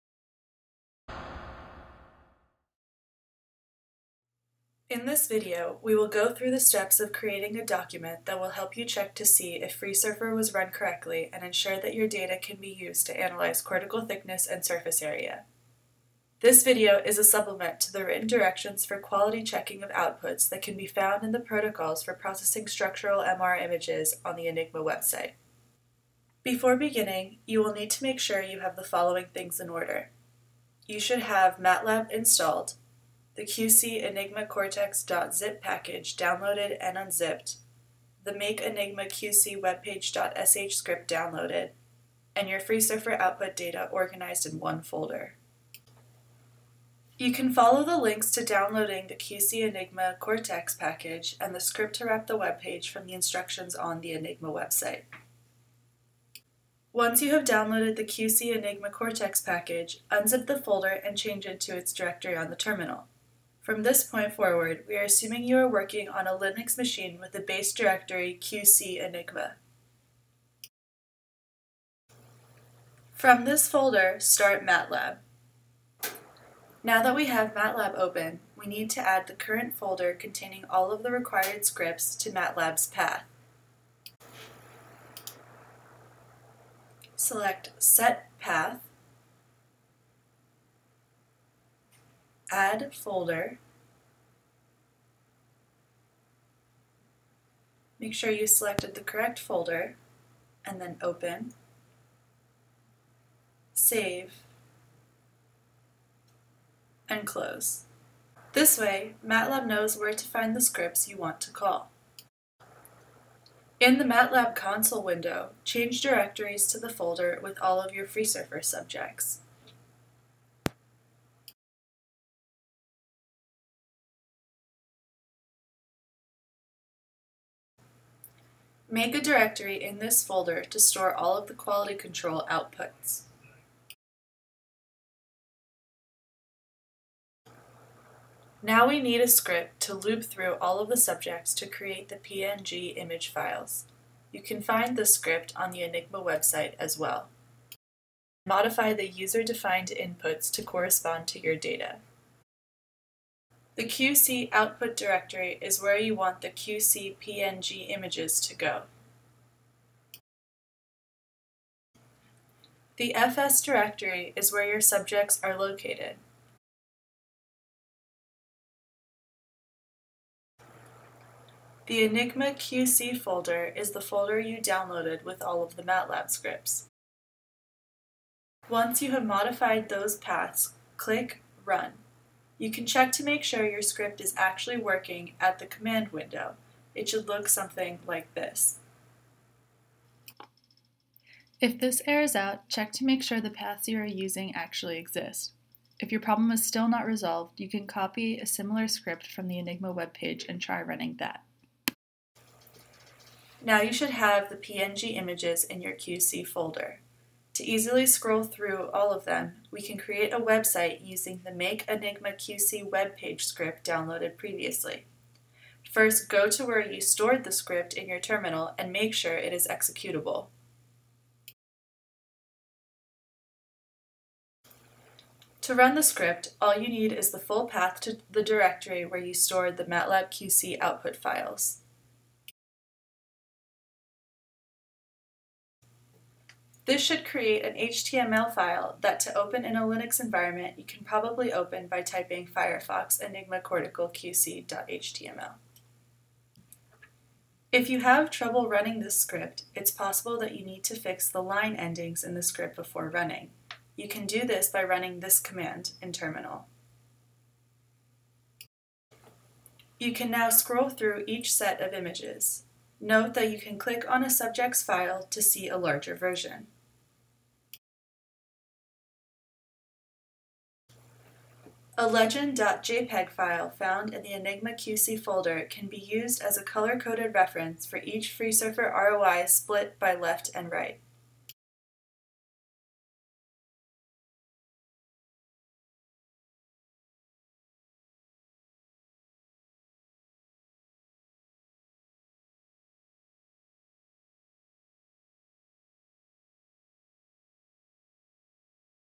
ENIGMA Image Processing: Cortical QC
A quick guide to the ENIGMA cortical quality control protocols using FreeSurfer.